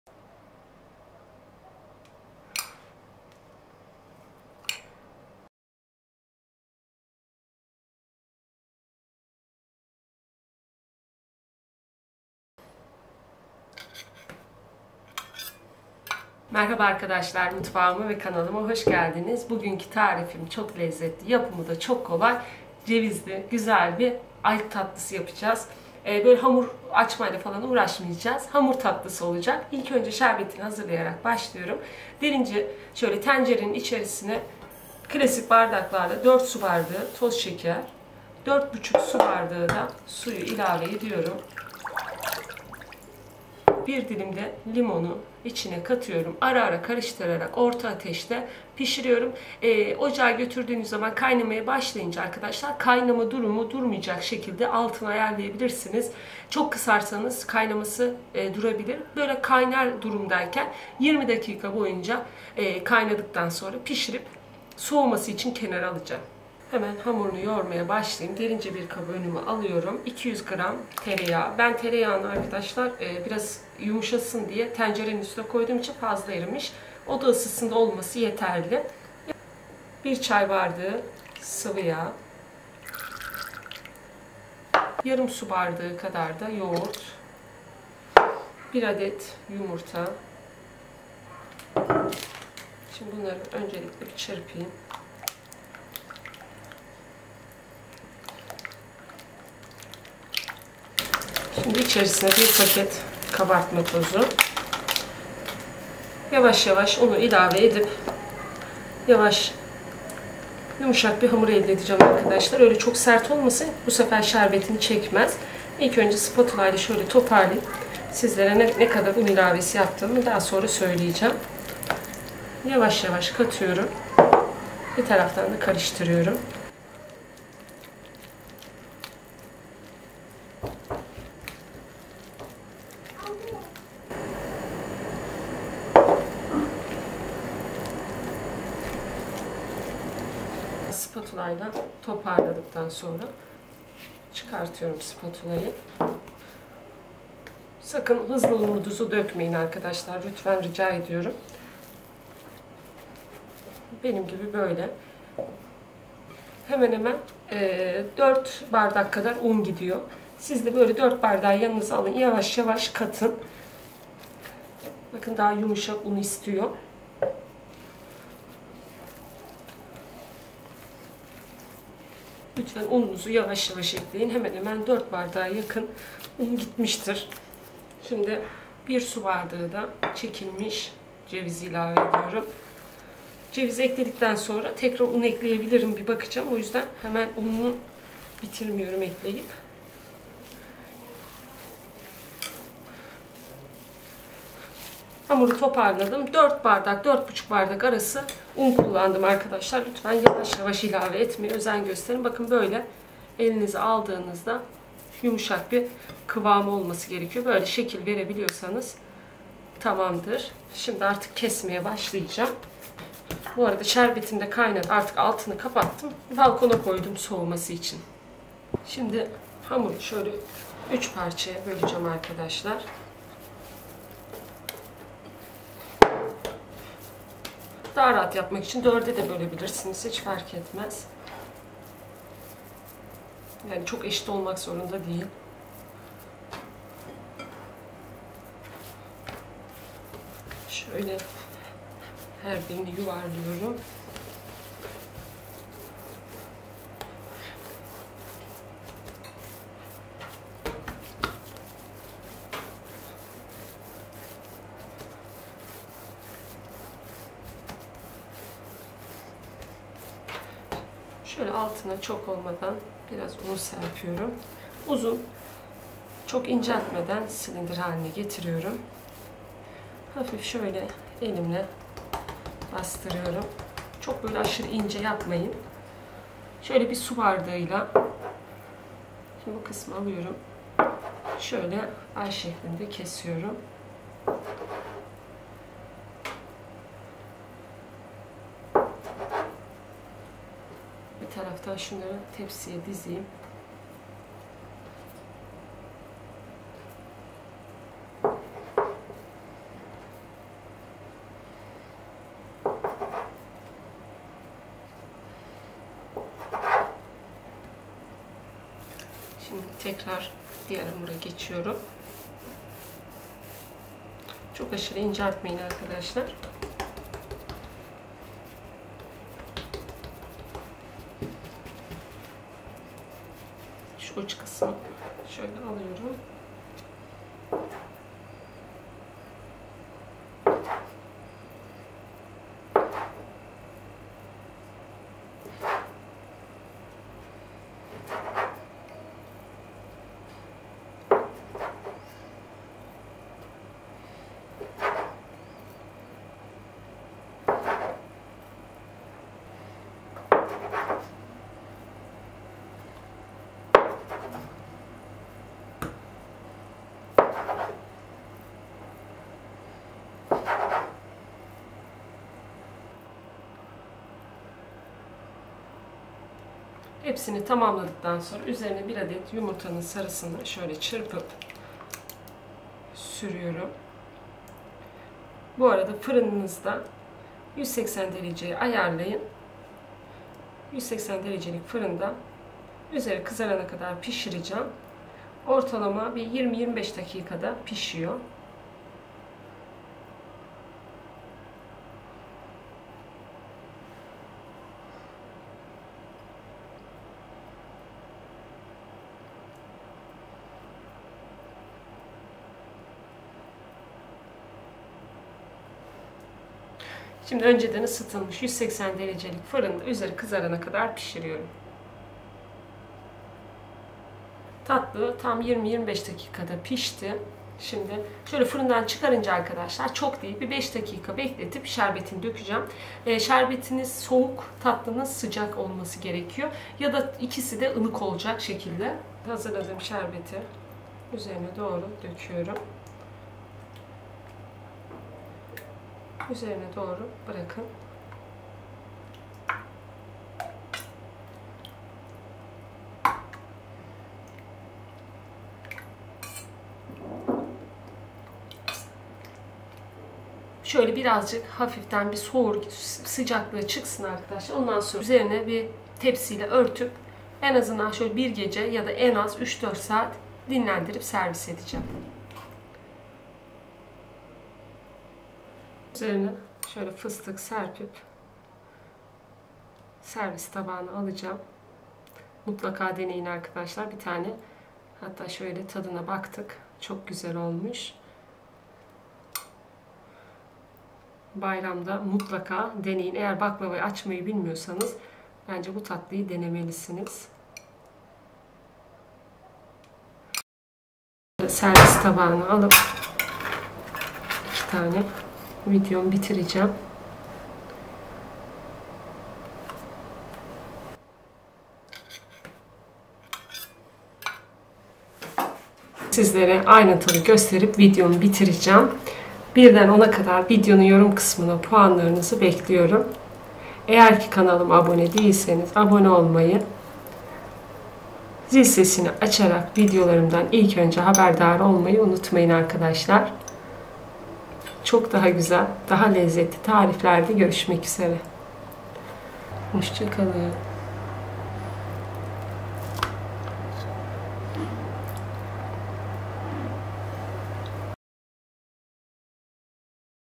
AY TATLISI‼️BU BAYRAMA BÖYLE KOLAY EFSANE TATLI YAPIN!!ŞERBETİ DÖKTÜĞÜNÜZ ANDA ÇEKİYOR!!
Merhaba arkadaşlar sizlere öyle bir tatlı tarifi hazırladım ki mutlaka denemenizi tavsiye ediyorum şerbetini anında çeken çok lezzetli bir tatlı yiyenler inanın ki lezzetine bayıldı özellikle herkes tarifini istedi hemen 10 dakikada yapabileceğiniz çok kolay bir ay tatlısı👍

MALZEMELER:
Şerbeti için:
‼️Ben 4 su bardağı kullandım fakat fazla geldi 3,5 bardak yeterli gelir

3,5 su bardağı şeker
4 su bardağı su
1 dilim limon
HAMURU İÇİN:
200 gr tereyağ yada margarin
1 çay bardağı sıvıyağ
Yarım su bardağı yoğurt
1 paket kabartmatozu
4 su bardağı un lütfen yavaş ekleyin yumuşak bir hamur elde edin yoksa şerbeti çekmez
1 su bardağı çekilmiş ceviz

‼️180 derecelik fırında 20 dakikada pişti sizde kendi fırın ayarınıza göre dikkatlice pişir üzerini kızardığını gördüğünüzde alabilirsiniz .￼